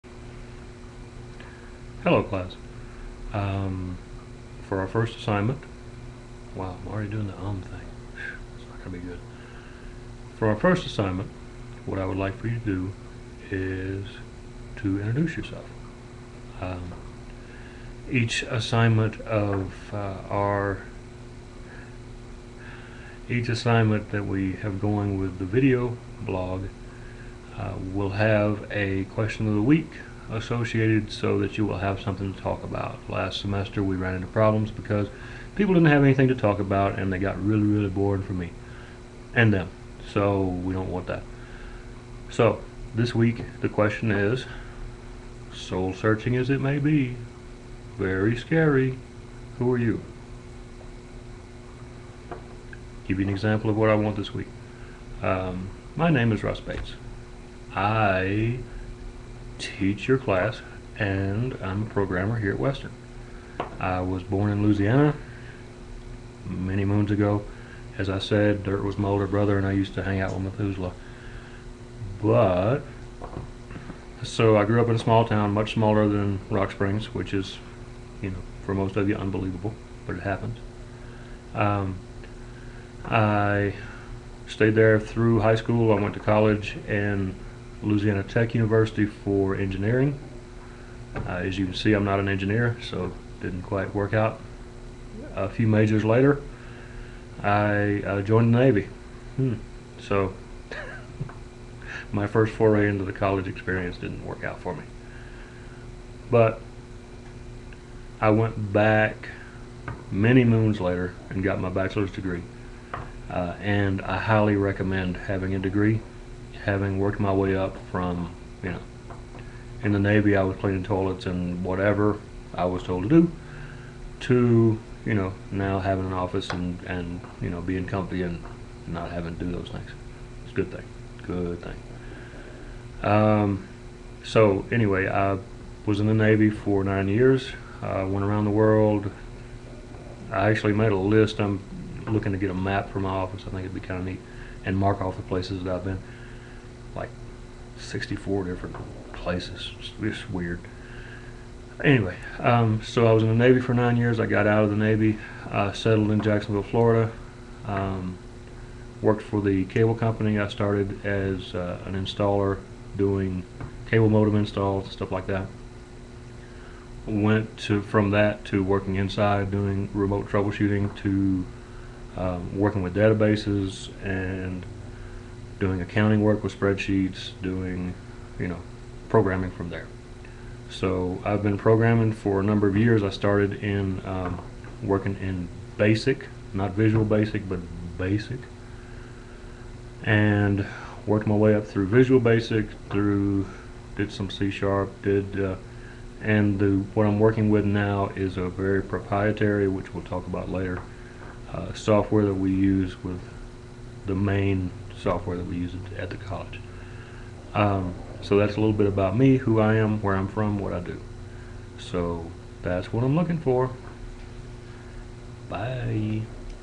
Spring 2012 Class Assignment 1 - Who are you?
An example of what I'm looking for in your first video - and a bit about me.